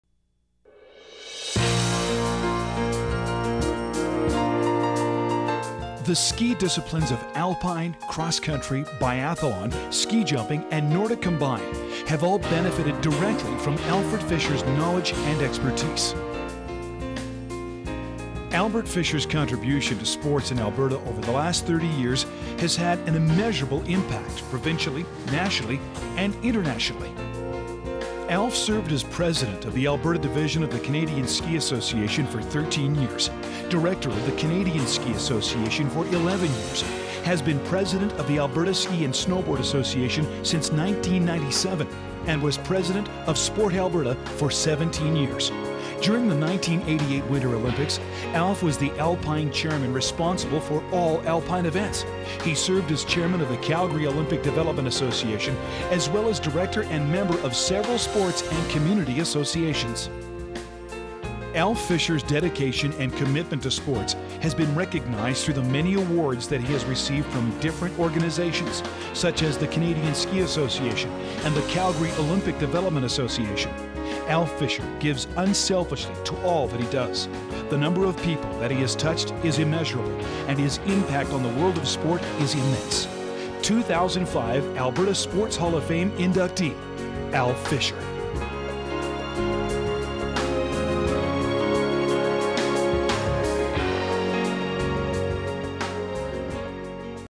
2005 Inductee - Alfred Fisher
2005 Builder - Multisport
Alfred Fischer had an immeasurable impact on the world of sports provincially, nationally and internationally.  He served as President of the Alberta Division of the Canadian Ski Association from 1983 to 1996, Director of the Canadian Ski Association from 1983 to 1994, and was President of the Alberta Ski and Snowboard Association starting in 1997.  During the 1988 Winter Olympics, Alfred Fischer was the Alpine Chairman responsible for all alpine events.  He served as Chairman of the Calgary Olympic Development Association (CODA), as well as director and member of several sports and community associations.